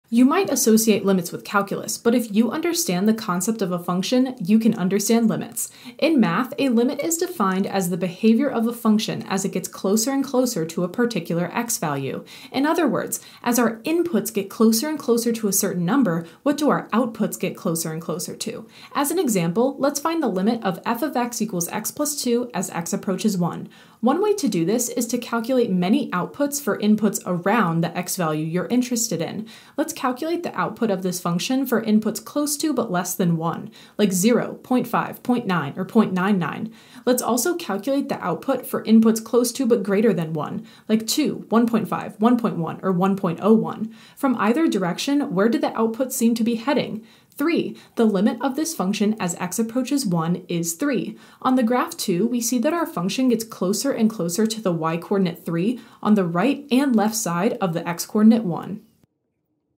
What is a limit? | AP Calculus | Calculus 1
Limits can seem scary, but they're directly related to our basic understanding of a function. Limits are just a newer idea based off of an older idea :)